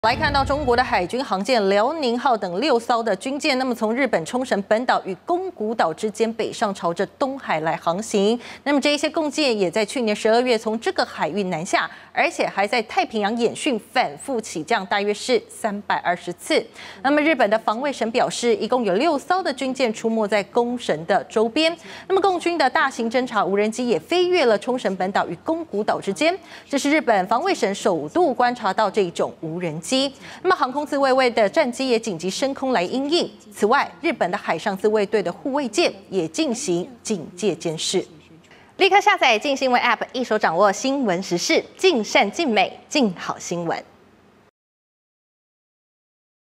6共艦出沒沖繩周邊、無人機飛越 日機艦警戒監視｜鏡新聞
中國海軍航艦遼寧號等6艘軍艦從日本沖繩本島與宮古島之間北上，朝東海航行。這些共艦也在去年12月從這個海域南下，並在太平洋演訓，反覆起降大約320次。